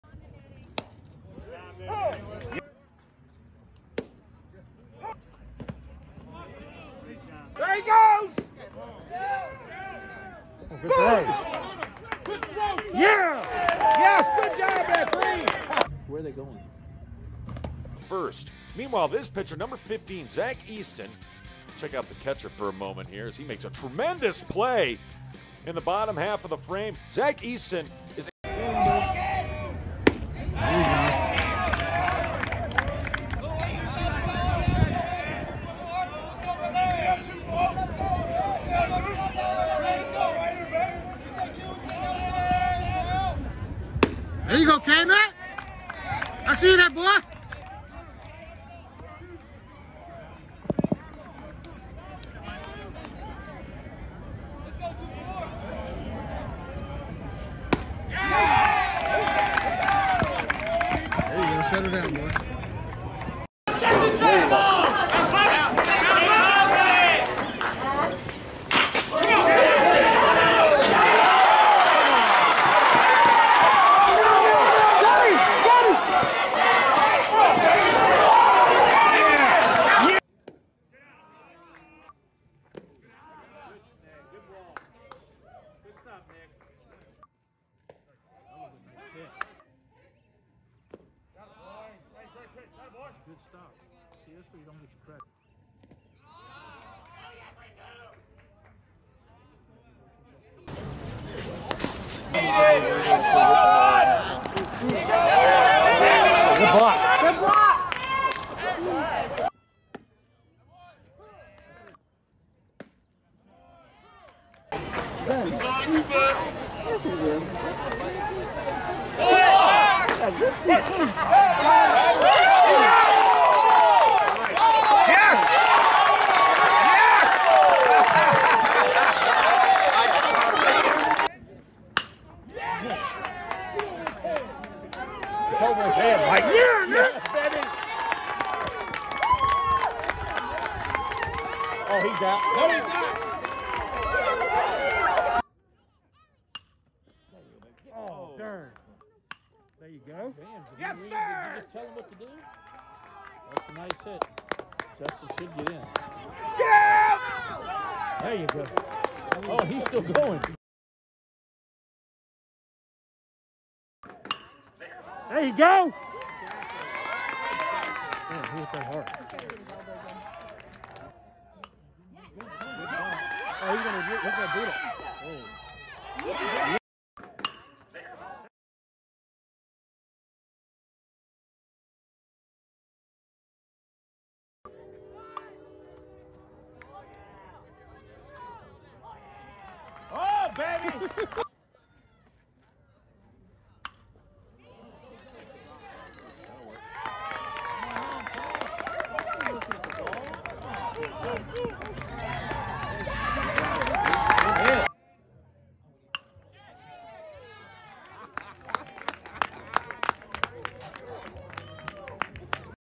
Nick McCray 2022 Catcher Hermitage High School/Huguenot Huskies 2022/2023
Nick McCray 2022 Catcher Richmond, VA
Highlights from first 2/3s of junior high school season. Spring 2021